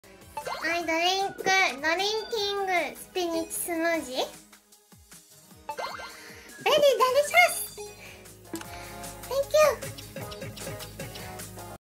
Tenshi drinks a spinach smoothie!
Very delicious!!!